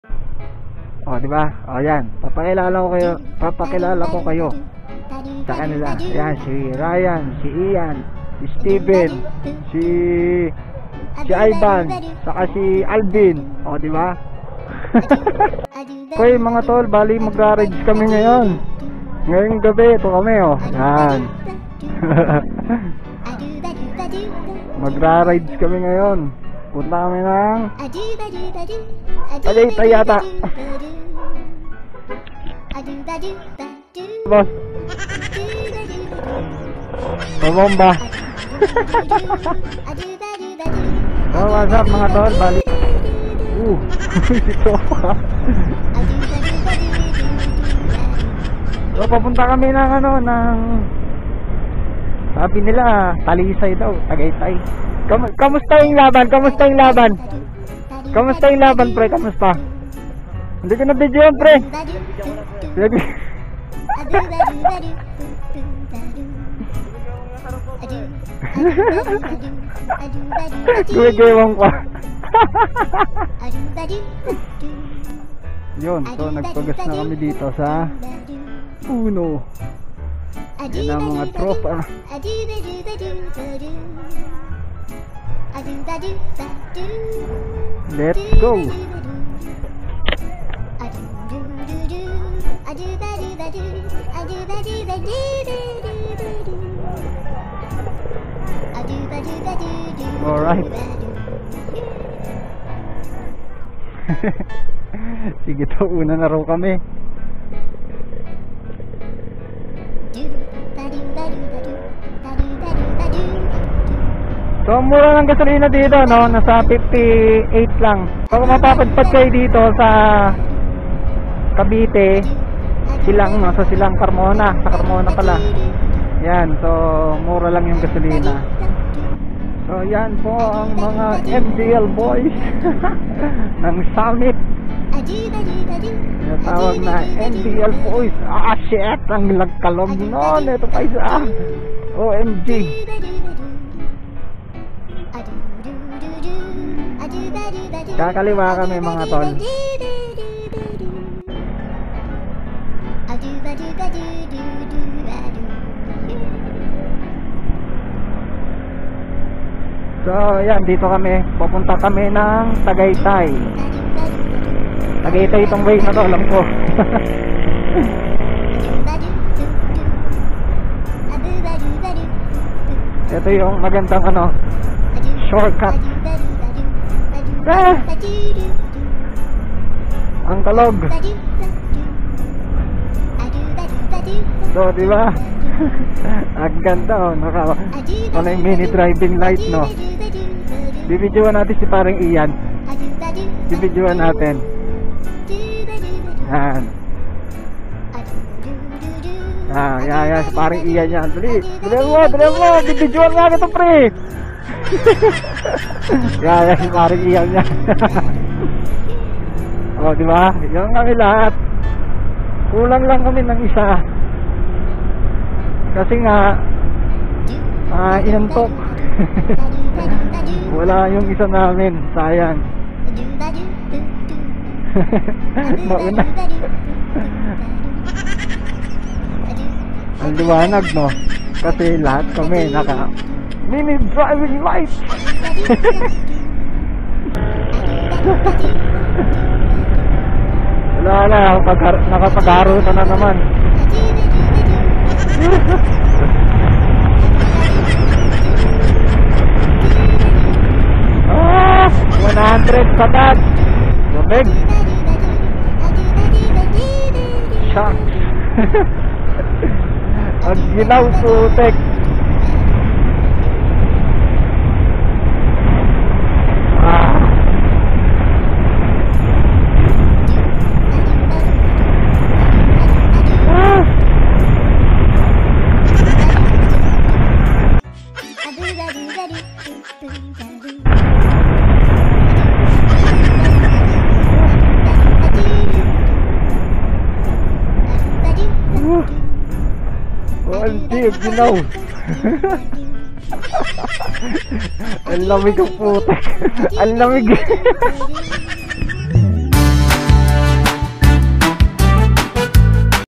Meron kaming Video Vlog P4
Rides namin Muntinlupa to tagaytay

Ano ginawa namin doon 😁
Ito ung link dito mo mapapanood